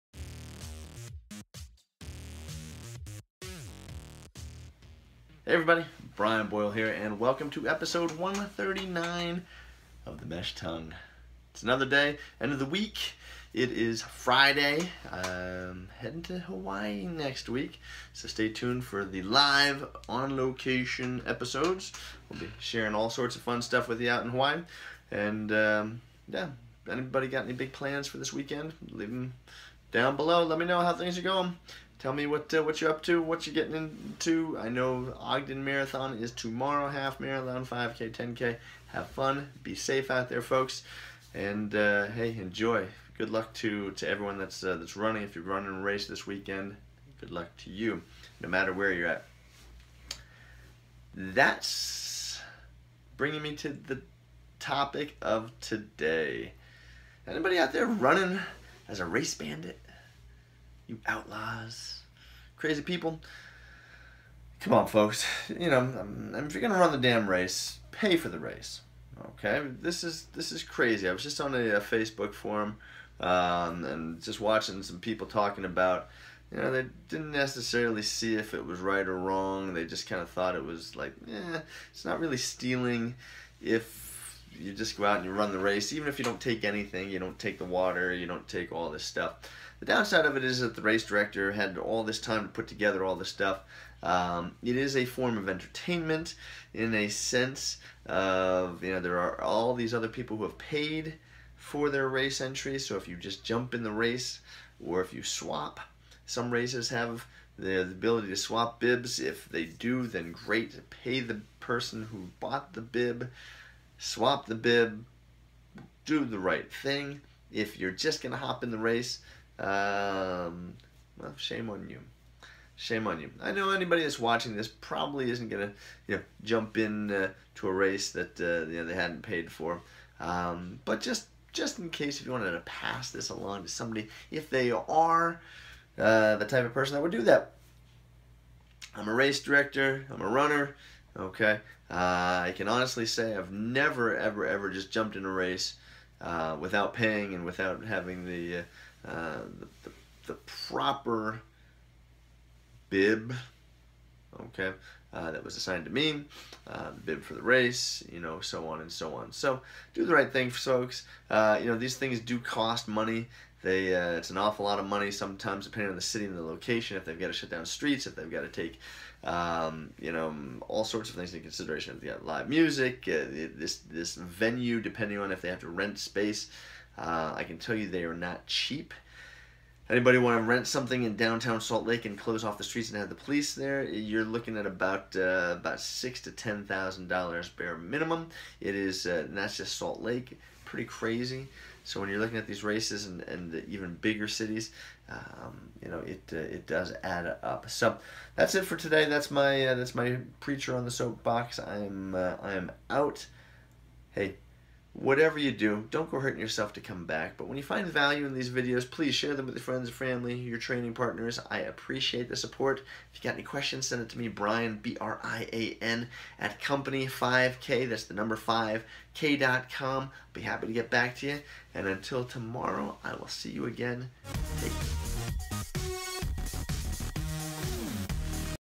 Race Bandits And Running: The Mesh Tongue Episode 139
In Episode 139 of the Mesh Tongue, I talk about Race Bandits. You know the people who participate in a race but don't cross the line and don't pay. Have you ever hopped in a race without paying for it? Don't be that guy or gal. Just my PSA for the day. What are your thoughts? Leave those comments below.